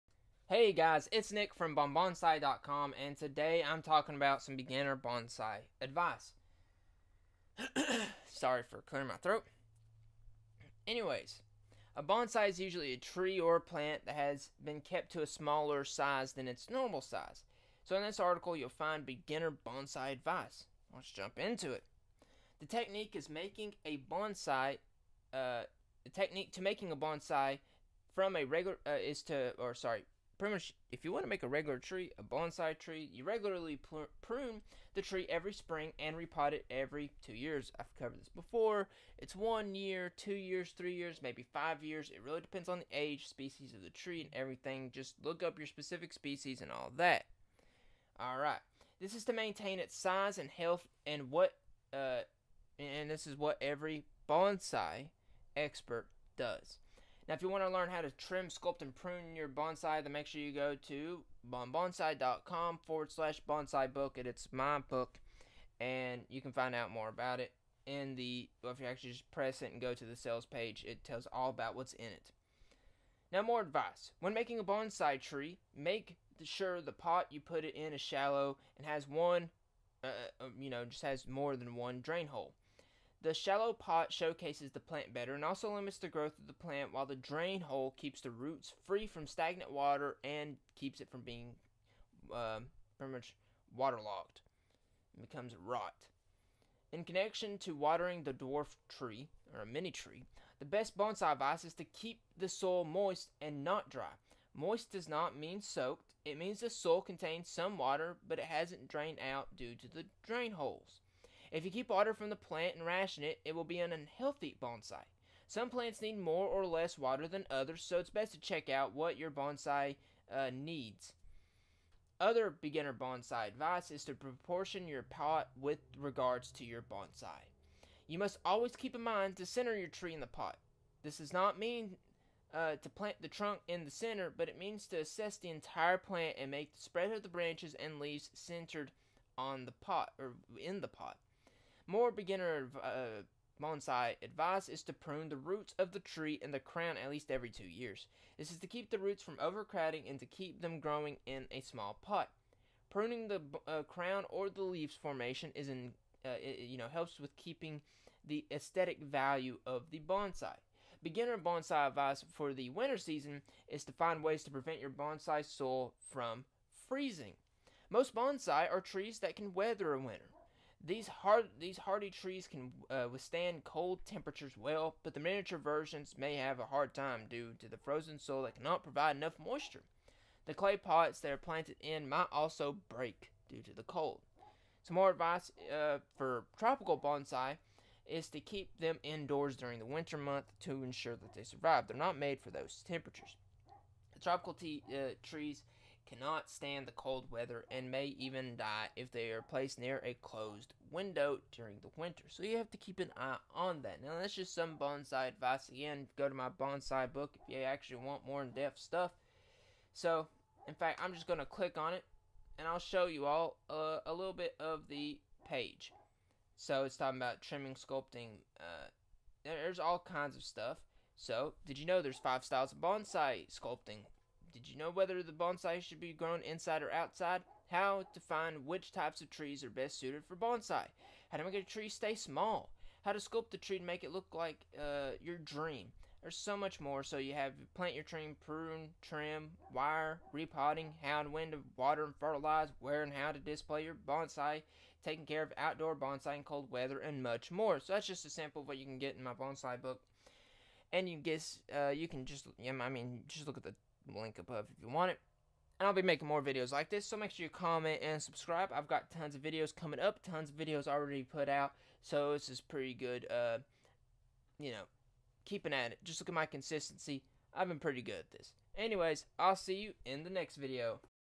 Beginner Bonsai Advice
About this video: Bonsai Advice For Beginners

Bonsai is usually a tree or plant that has been kept smaller than its normal size. The technique to making a bonsai plant is to regularly prune the tree every spring and to repot it every two years. This is to maintain its size and health and what every bonsai expert does.

More Bonsai Advice

When making a bonsai tree plant, make sure the pot you put it in is shallow and has more than one drain hole. The shallow pot showcases the plant better and also limits the growth of the plant while the drain holes keep the roots free from stagnant water and water rot.

In connection to watering the dwarf tree, the best bonsai advice is to keep the soil moist and not dry. Moist does not mean soaked, it means the soil contains some water but has drained out due to the drain holes. If you keep water from the plant and ration it, it will be an unhealthy bonsai. Some plants do need more or less water than others, so it is better to check out what bonsai tree you have to be able to apply the bonsai advice properly.

Other good bonsai advice for beginners is to proportion your pot with regards to the bonsai tree. You must always keep in mind to center your tree in the pot. This does not mean to plant the trunk in the center but it means to assess the entire plant and make the spread of the branches and the leaves centered on the pot.

More bonsai advice is to prune the roots of the tree and the crown at least every two years. This is to keep the roots from overcrowding and to keep them growing in the small pot. Pruning the crown or the leaves formation is in keeping to the aesthetic value of the bonsai plant.

Bonsai advice for the winter season is to find ways to prevent your bonsaiís soil from freezing. Many bonsai trees are trees that can weather a winter. These hardy trees can withstand cold temperatures well but the miniature versions may have a hard time due to frozen soil that cannot provide enough moisture. The clay pots they are planted in might also break due to the cold.

Bonsai advice for tropical bonsai plants or trees is to keep them indoors during the winter months to ensure they survive. These tropical trees cannot stand the cold weather and can die even if they are placed near a closed window during winter.